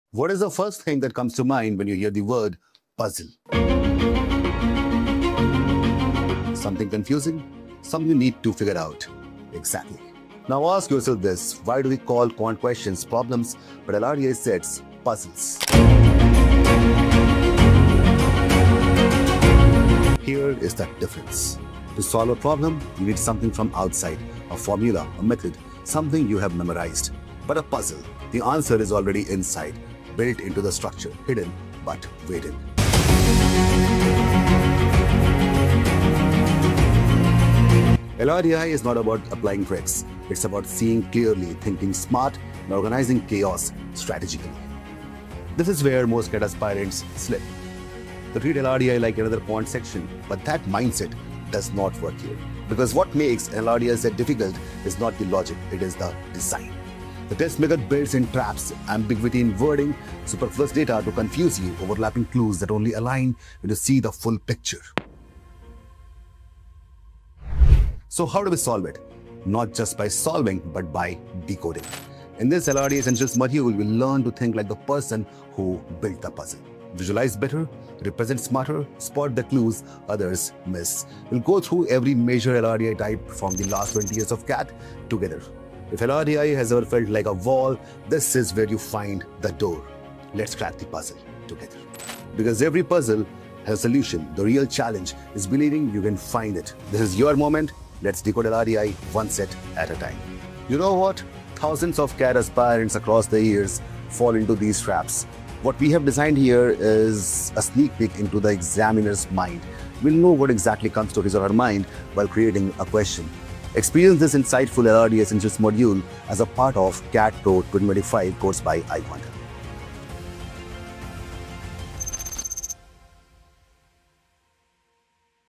The Visualization Secret Top 1% Know | LRDI Essentials by iQuanta | CAT 25 New batch Launched!
🟢Enroll for iQuanta CAT New Batch

Visualization is the invisible skill that separates CAT toppers from the rest.

It’s not about solving faster — it’s about seeing patterns others miss.

In this cinematic video, discover how LRDI Essentials by iQuanta will train you to think, visualize, and solve puzzles like a true 99%iler.

✅ What you’ll learn inside LRDI Essentials:
🔹 How to decode any Logical Reasoning or Data Interpretation set through visualization
🔹 The structured thinking patterns CAT toppers use
🔹 Techniques to solve unseen and unpredictable LRDI sets in CAT

📢 New Batch for CAT 2025 is LIVE!
If you’re serious about cracking LRDI and boosting your CAT score, now is the time to start.

➔ Join the LRDI Essentials journey today and change the way you think forever.